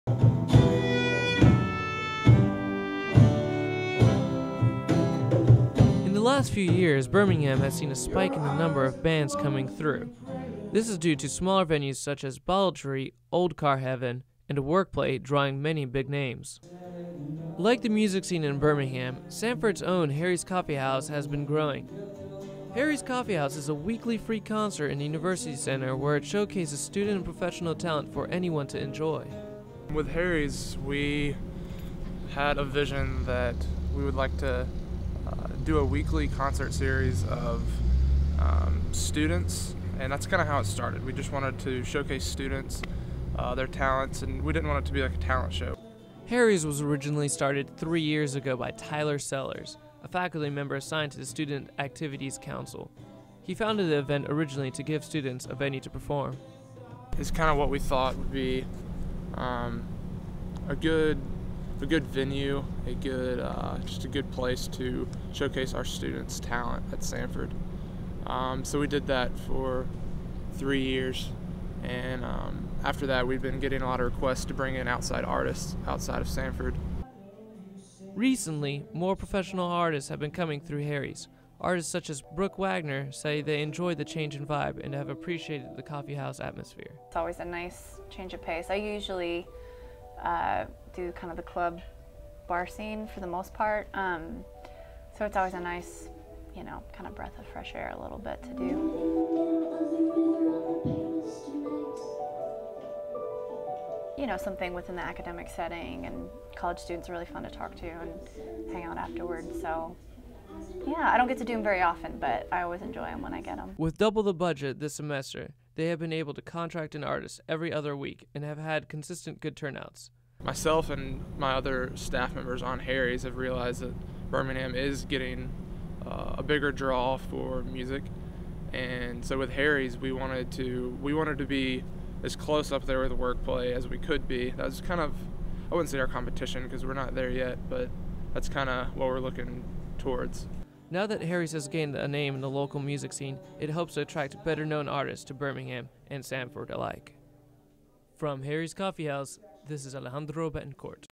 Harry's Coffee House
SNN culture reporter, Alejandro Betancourt, takes a look into Samford University's Harry's Coffee house and the growing of number of music artists coming through Samford.